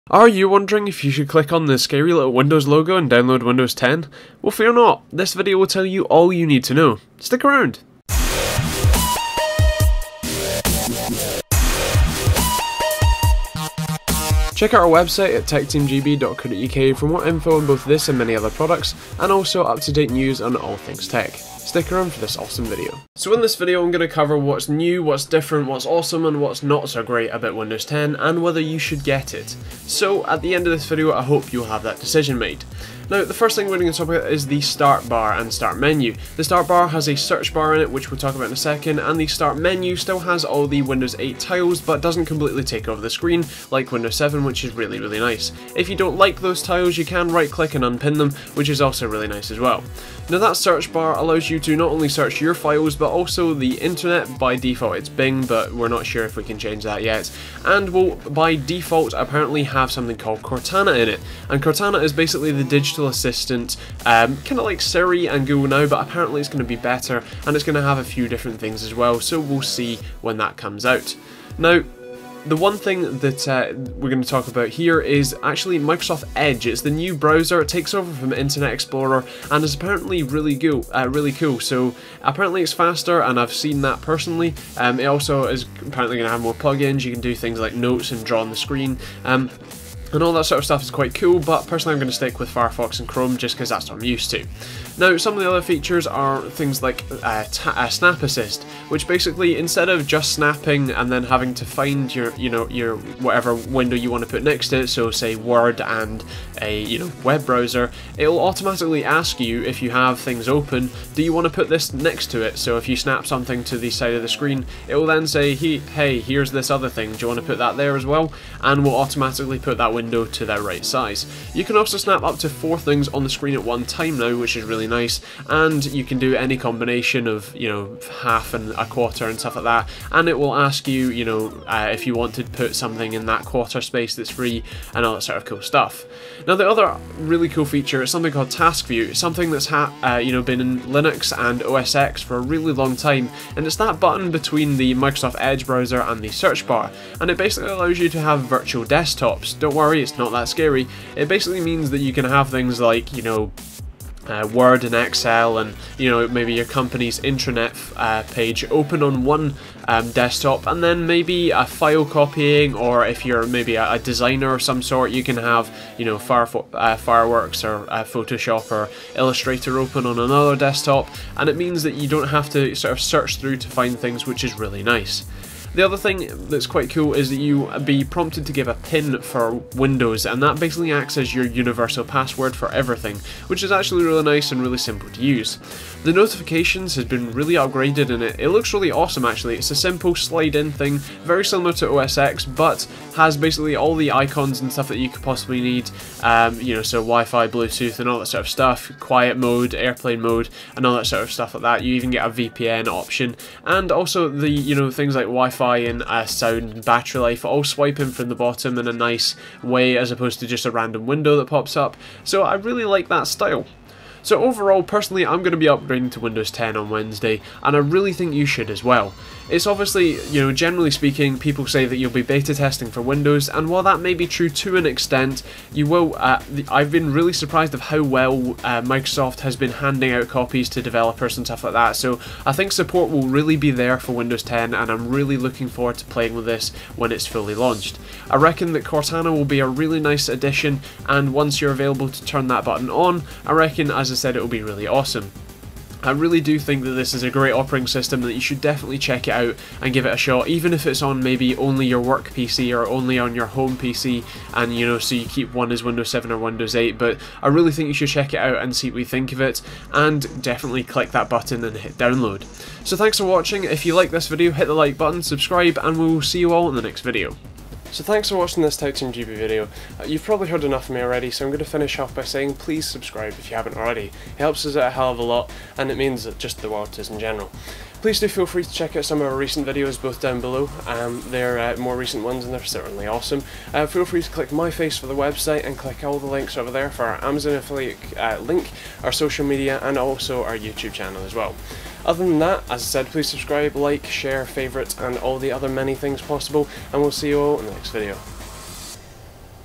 Should I Get Windows 10?
Windows 10 was announced exactly 10 months ago today, now its finally been released your probably wondering whether or not you should jump on board with the update. In this video we're going to give you all the information you need to know to make the right decision.